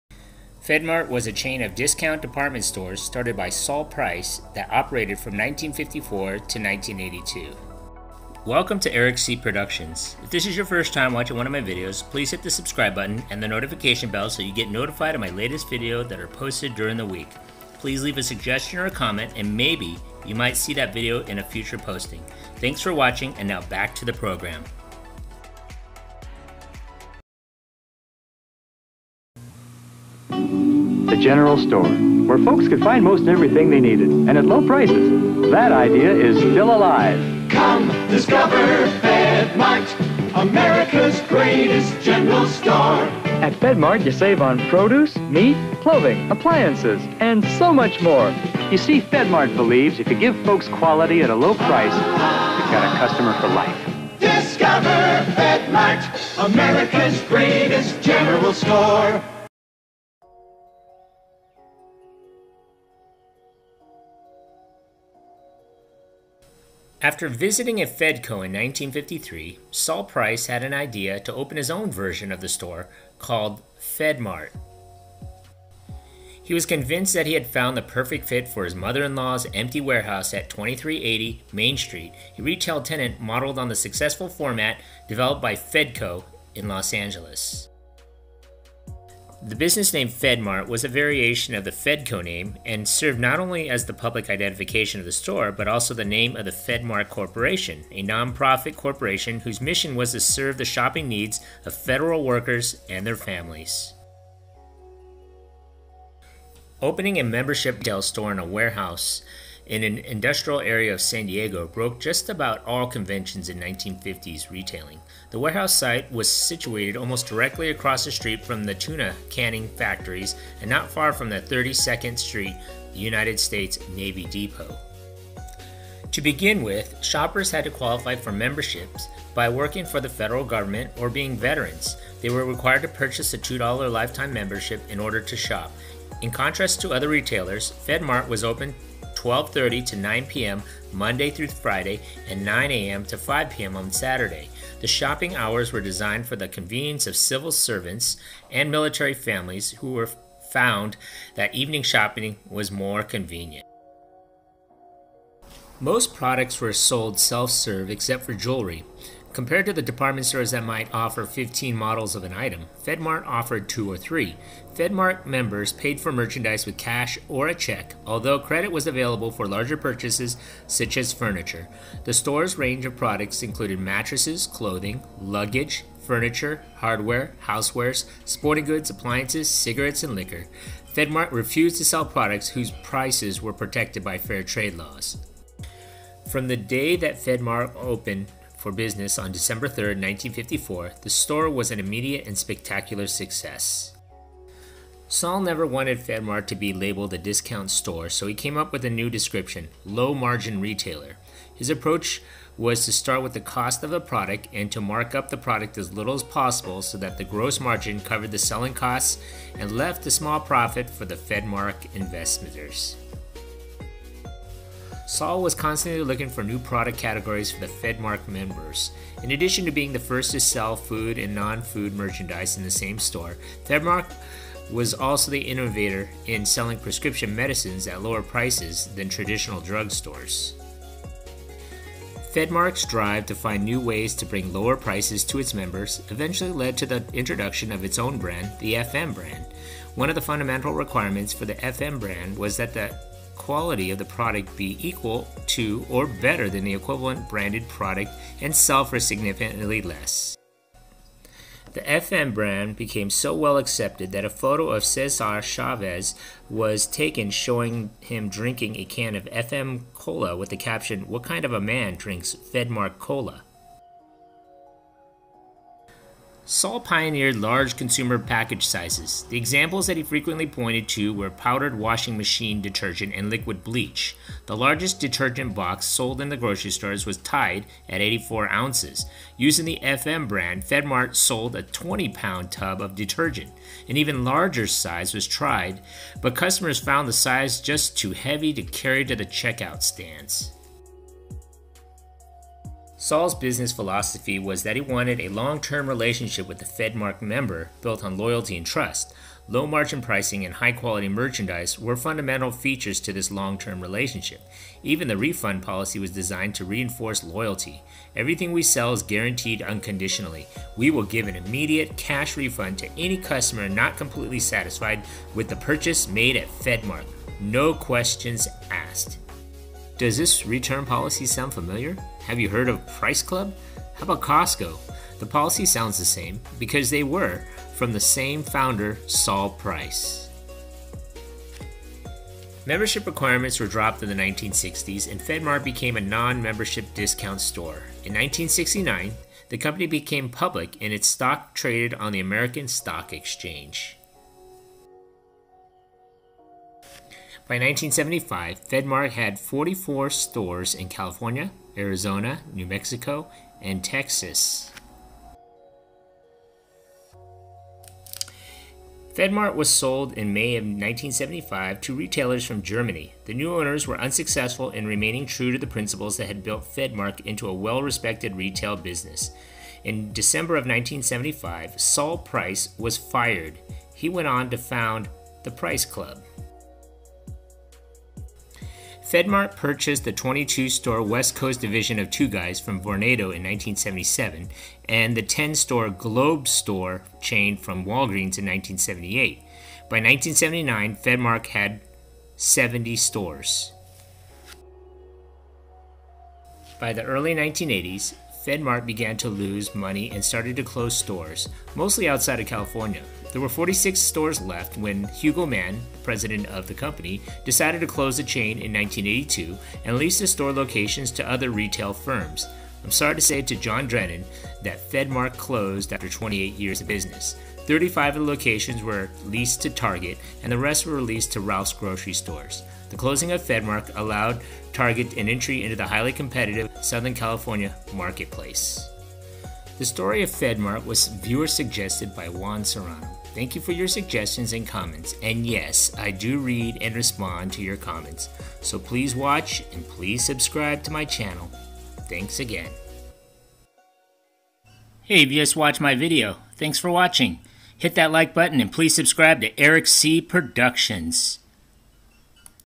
Do You Remember Fedmart? A Store History.
FedMart was a chain of discount department stores started by Sol Price, who later founded Price Club. His first location in San Diego, California, was in a converted airport hangar. It was originally a discount department store open to government employees, who paid a membership fee of $2 per family. FedMart's first year was highly successful. Over the next 20 years Fedmart grew to include 45 stores in a chain that generated more than $300 million in annual sales. The business expanded to several states in the Southwest United States. Many stores were previous White Front or Two Guys locations. Price later sold two-thirds of the chain to Hugo Mann, a German retail chain, in 1975 and was forced out of his leadership position the following year. FedMart went out of business in 1982.
Here is a brief history about the company that began in 1954 and closed in 1982.